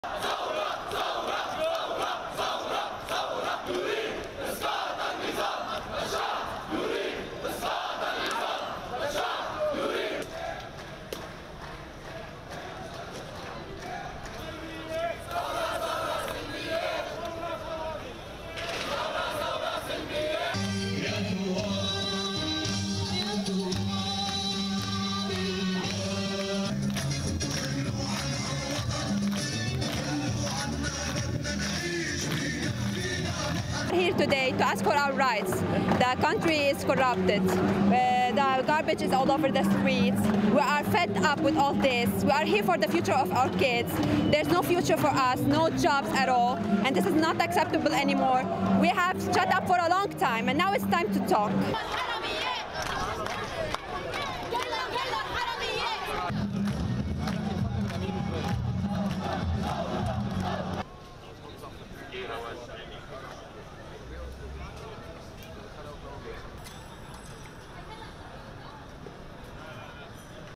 Protest in Beirut over tax hike plan
(18 Oct 2019) Demonstrators on Friday blocked major roads across Beirut in a second day of protests against proposed new taxes, which come amid Lebanon's severe economic crisis.
Riot police watched on as hundreds of people gathered near the office of Prime Minister Saad Hariri, some waving Lebanese flags.
The protests were sparked when the government announced plans for a suite of new taxes including voice calls made through messaging applications such as Whatsapp.
The protests could plunge Lebanon into a political crisis with unpredictable repercussions for the economy, which has been in steady decline.